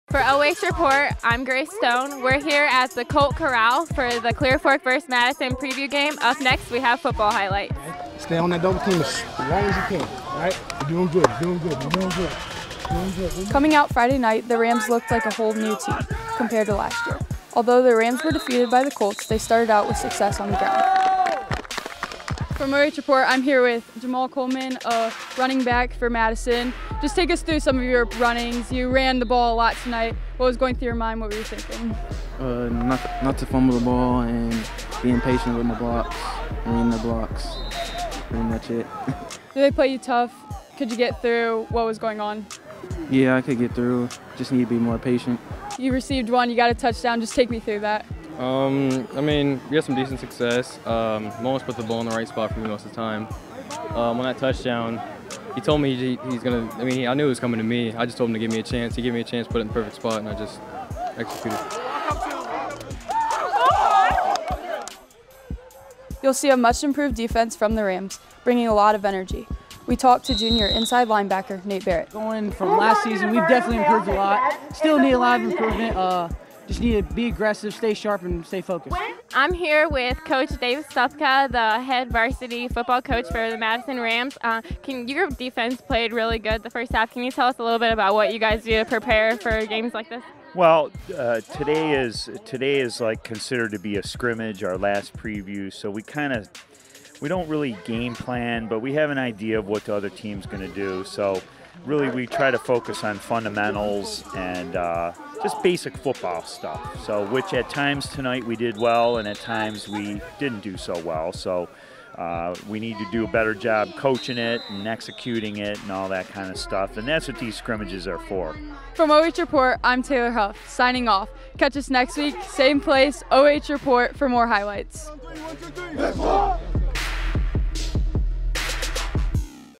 All-Access Madison Football Preview Scrimmage
Under second-year head coach Dave Stupka, the Madison football program is looking to make some strides on the gridiron this season. With a new philosophy now in place, the Rams will try and improve on last year's 1-9 showing after finishing up their preseason last week.

Join WMAD reporters Grayce Stone and Taylor Huff as they take you on the sideline and into the action at the Colt Corral for Madison's preview scrimmage against Clear Fork. The Rams would go on to lose their dress rehearsal by a score of 28-7, but the team showed progress as they approach the regular season.

​Madison will host Shelby on Friday night to kick off the 2019 season.